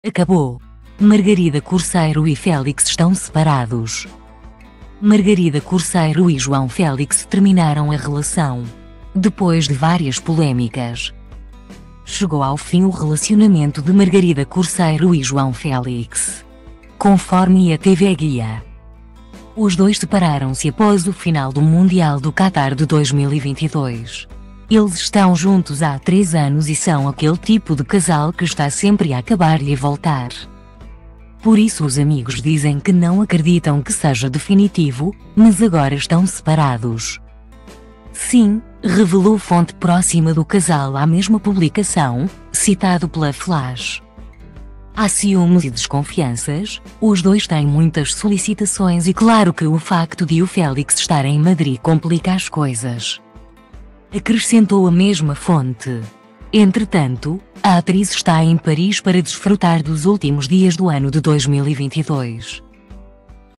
Acabou! Margarida Corceiro e Félix estão separados
Margarida Corceiro e João Félix terminaram a relação. Depois de várias polémicas,chegou ao fim o relacionamento de Margarida Corceiro e João Félix. Conforme a TV Guia,os dois separaram-se após o final do Mundial do Qatar de 2022. "Eles estão juntos há três anos e são aquele tipo de casal que está sempre a acabar e a voltar,